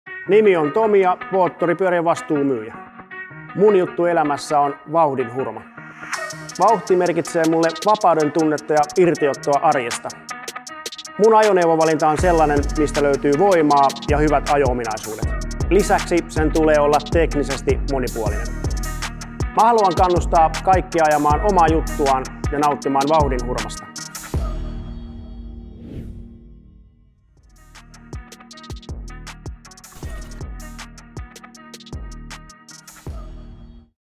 Aja Omaa Juttuasi: Tomi Jakobsson | J. Rinta-Jouppi
🗣️: "Mun juttu elämässä on vauhdin hurma. Vauhti merkitsee mulle vapauden tunnetta ja irtiottoa arjesta."

💬 Tomi Jakobsson, moottoripyörien vastuumyyjä